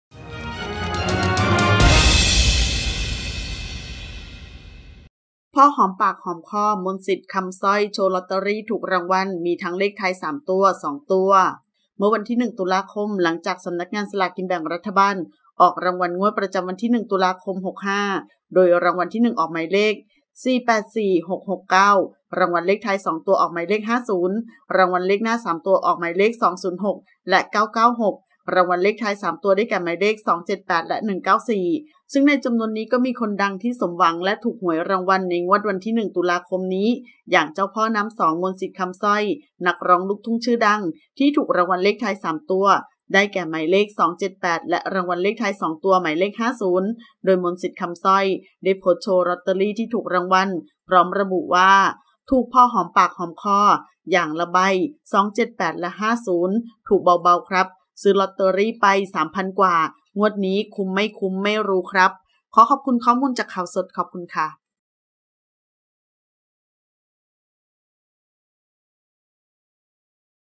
พอหอมปากหอมคอ! “มนต์สิทธิ์” โชว์ลอตเตอรี่ถูกรางวัล มีทั้งเลขท้าย 3 ตัว 2 ตัว
เมื่อวันที่ 1 ต.ค.65 หลังจากสำนักงานสลากกินแบ่งรัฐบาล ออกรางวัลงวดประจำวันที่ 1 ต.ค.65 โดยรางวัลที่ 1 ออกหมายเลข 484669 รางวัลเลขท้าย 2 ตัว ออกหมายเลข 50 รางวัลเลขหน้า 3 ตัว ออก 206 และ 996 รางวัลเลขท้าย 3 ตัว ออก 278 และ 194

ในจำนวนนี้มีคนดังที่สมหวังและถูกหวยรวยรางวัลในงวดวันที่ 1 ต.ค.65 อย่างพ่อน้ำสอง มนต์สิทธิ์ คำสร้อย นักร้องลูกทุ่งชื่อดัง ที่ถูกรางวัลเลขท้าย 3 ตัว ออก 278 และรางวัลเลขท้าย 2 ตัว หมายเลข 50

โดยมนต์สิทธิ์โพสต์โชว์ลอตเตอรี่ที่ถูกรางวัล พร้อมระบุว่า “ถูกพอหอมปากหอมคอ อย่างละใบ 278-50 ถูกเบาๆ ครับ ซื้อลอตเตอรี่ไป 3 พันกว่า งวดนี้ คุ้มไม่คุ้ม ไม่รู้ครับ”

00:00 - Intro
00:10 - News
01:00 - Minute